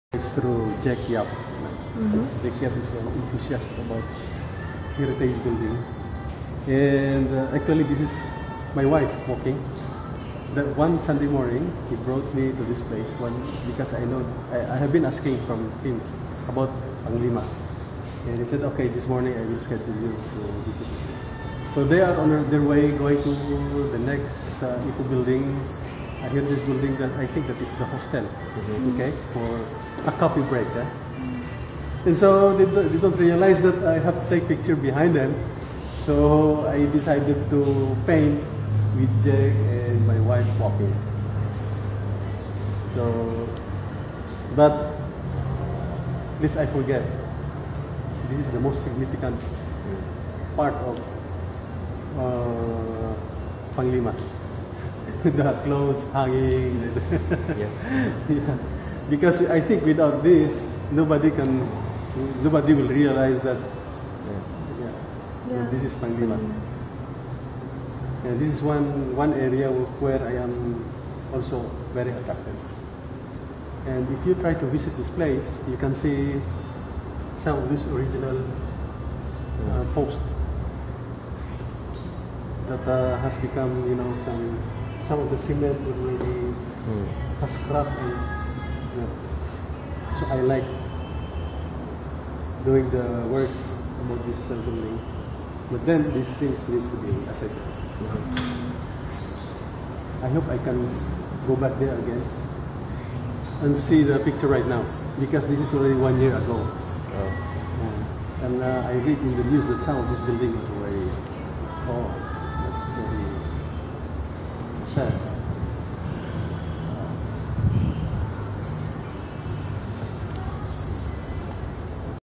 Mr Julbert Maranon explains the story behind his Panglima Lane painting.
A solo Art Exhibition of the work of Mr Julbert Maranon